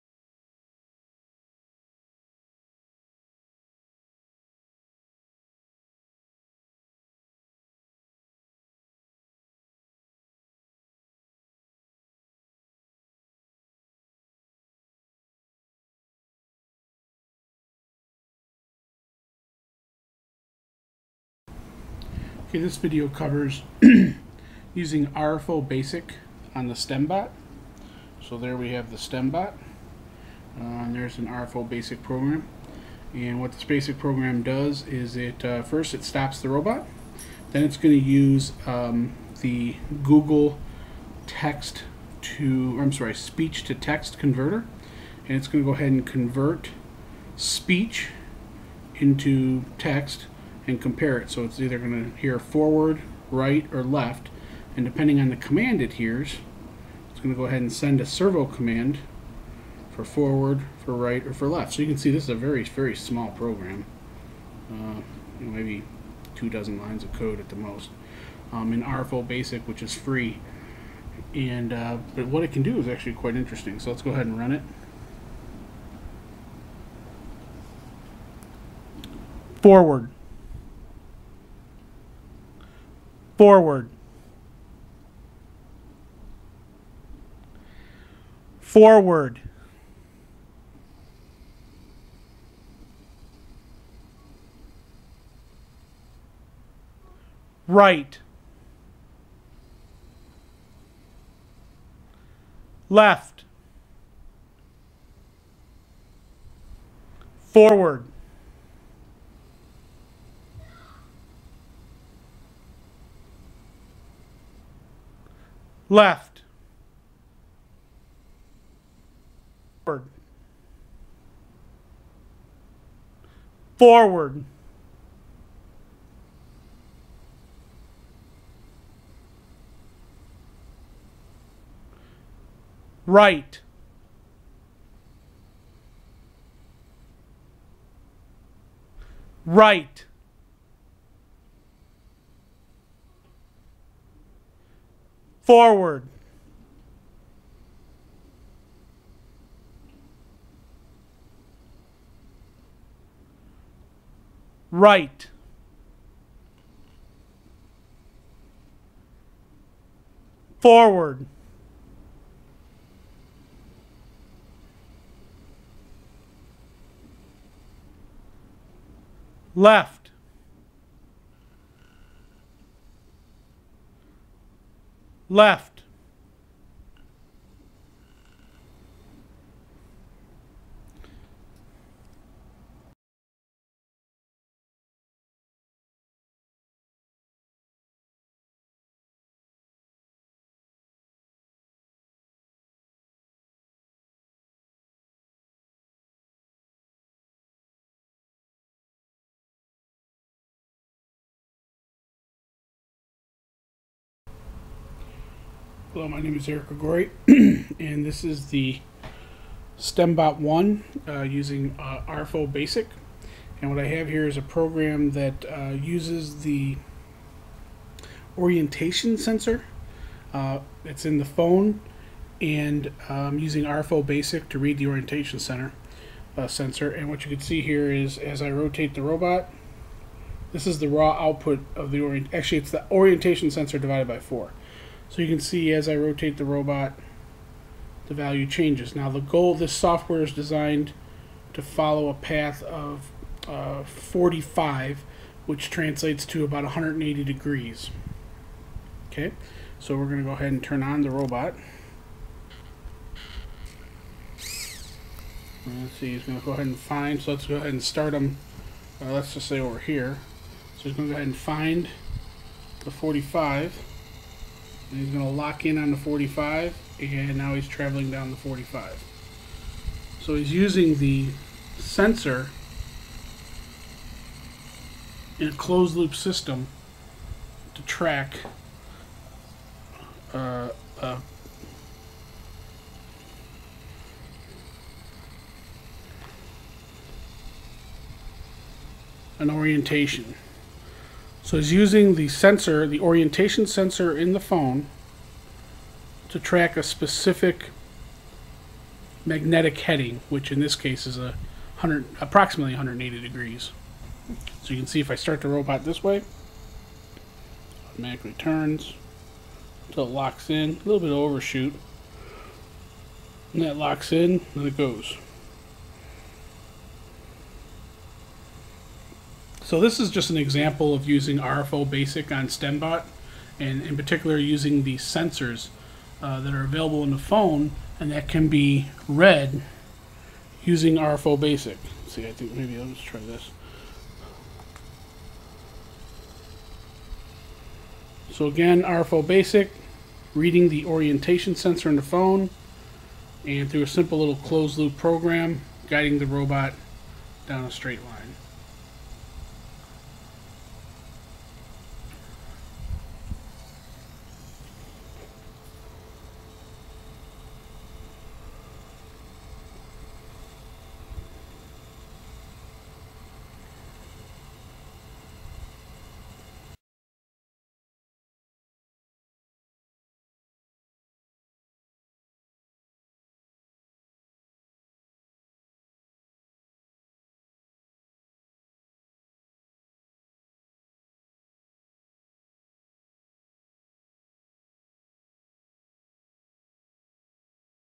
Using RFO BASIC with STEMbot1 to Create a Voice Controlled Robot and a Sensor Controlled Robot
RFO BASIC is an easy to use BASIC programming language for Android designed by the guy who created Atari BASIC RFO BASIC is open source, so I added a command to control STEMbot1. Simply download my modified version of RFO BASIC You can find the source for the EMGRobotics RFO BASIC 'servo' command

This video shows just a few of the many features available in RFO BASIC, speech to text and reading sensors. The RFO BASIC command 'stt' is used to listen for a command, convert the command to a BASIC string, and save the string in a BASIC variable. each command, 'forward', 'right', 'left' execute a different 'servo' command to control the robot.

RFO BASIC supports most of the sensor available in an Android phone, including the orientation sensor used in this video. The orientation sensor combines magnetic, accelerometer, and gyro data to create robust data representing the orientation of the phone. This data is used to guide the robot in a straight line.